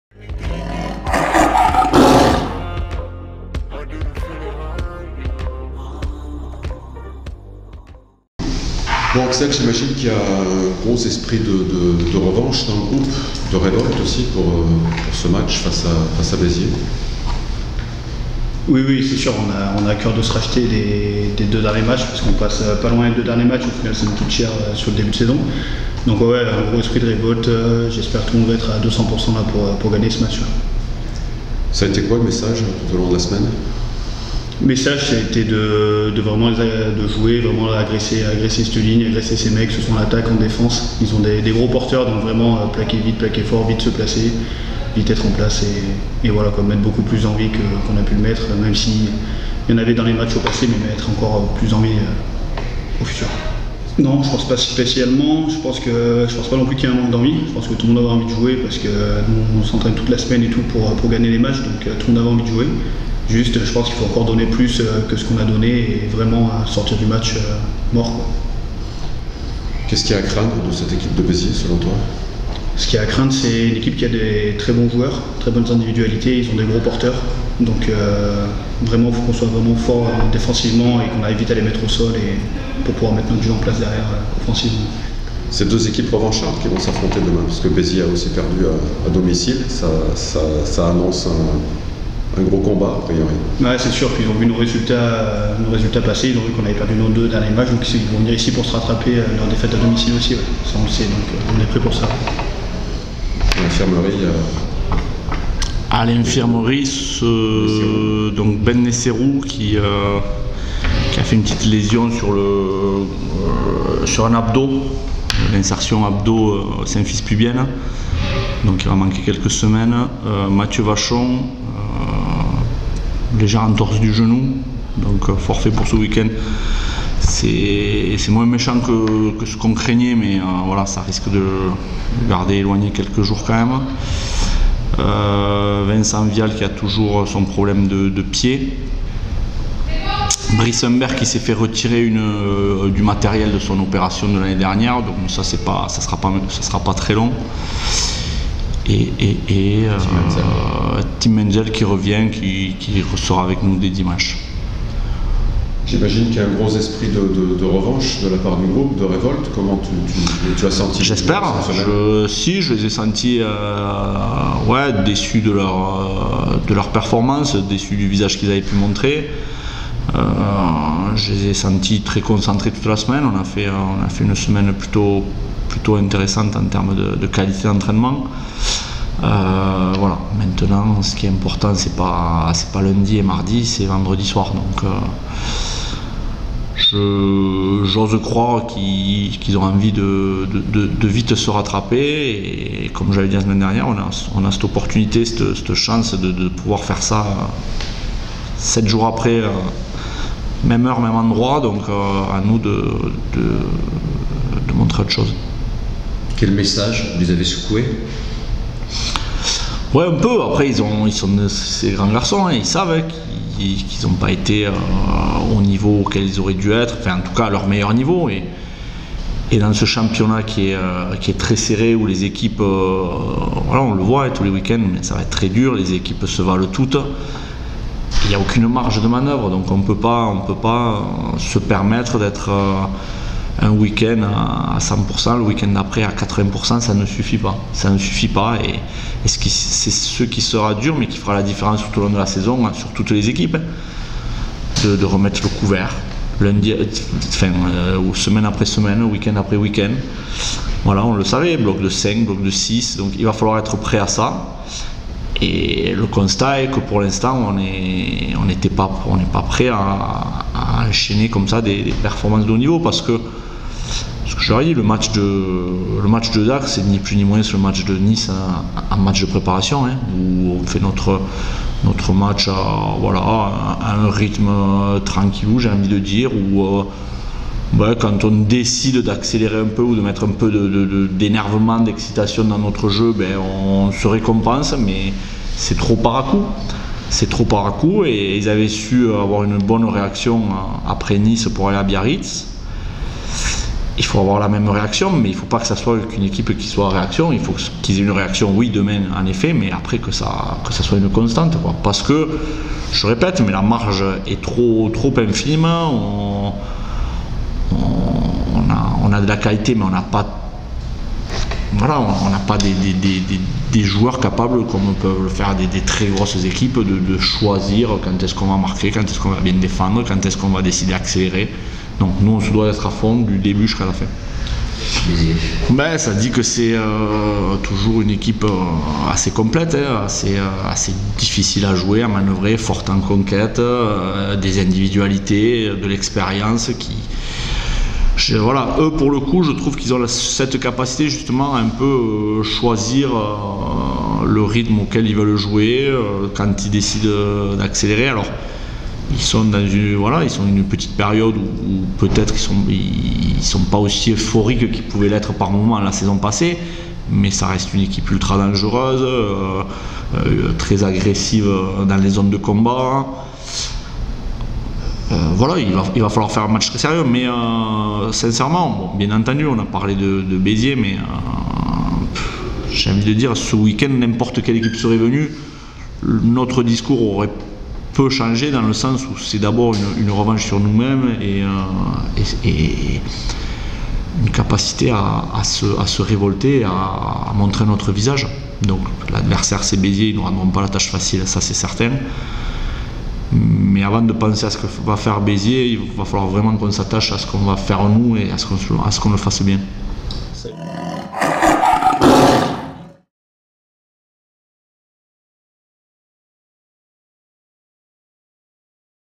𝐒𝐀𝐈𝐒𝐎𝐍 𝟐𝟒/𝟐𝟓 - J.03 - VRDR VS ASBH
Retrouvez la conférence de presse avec Fabien et notre capitaine Axel avant le match face à Béziers !